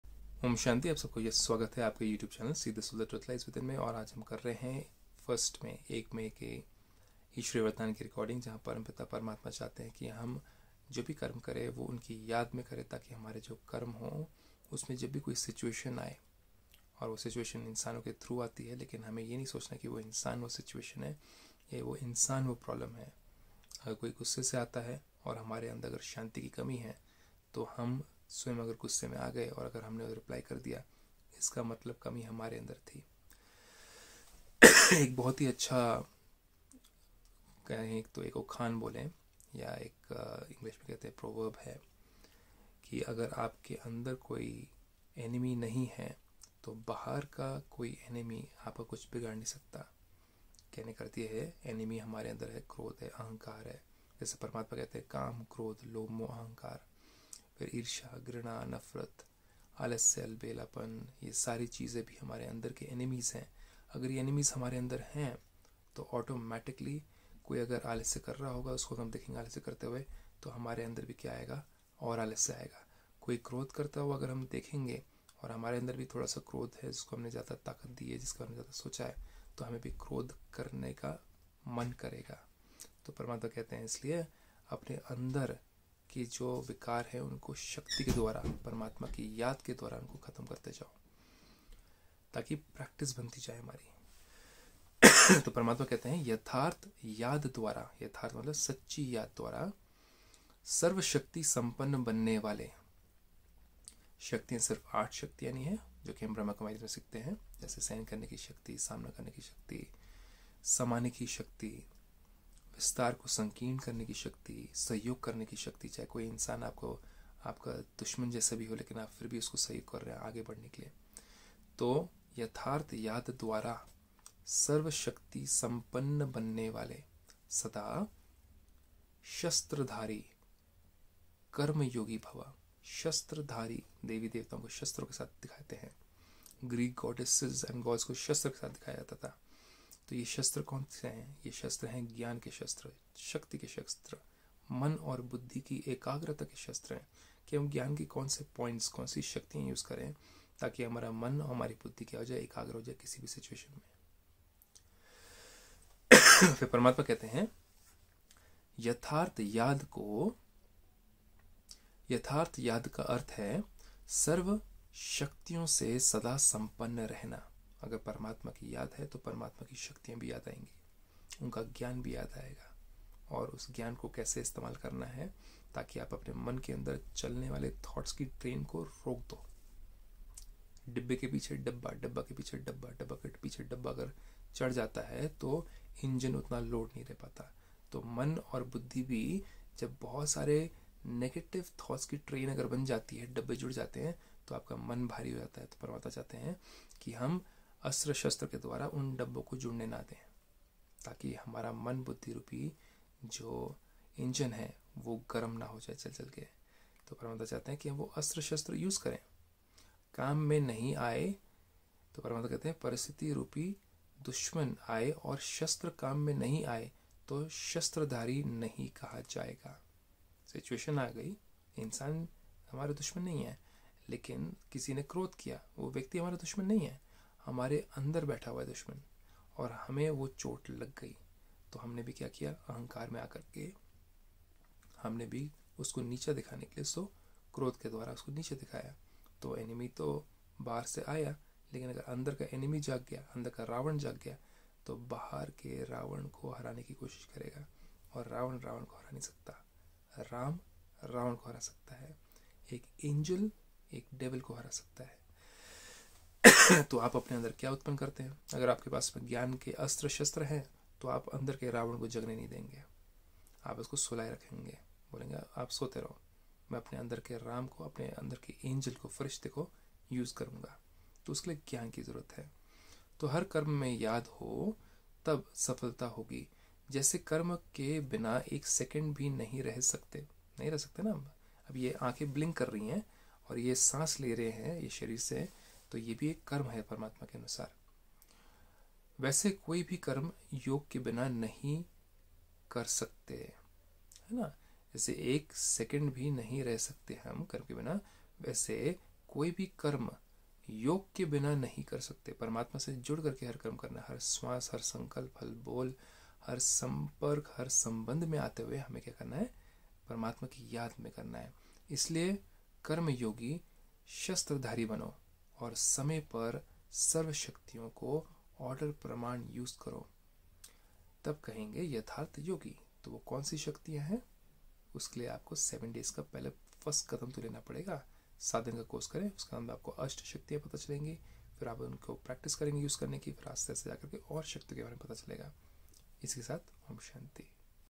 प्रत्येक कर्म को भगवान की याद के साथ करने से हमें किसी भी प्रतिकूल स्थिति का सामना करने के ...
दिन-90 प्रत्येक कर्म को भगवान की याद के साथ करने से हमें किसी भी प्रतिकूल स्थिति का सामना करने के लिए सही समय पर 8 ईश्वरीय शक्तियों का उपयोग करने में मदद मिलती है।

ओम शांति
दिव्य प्रेम परम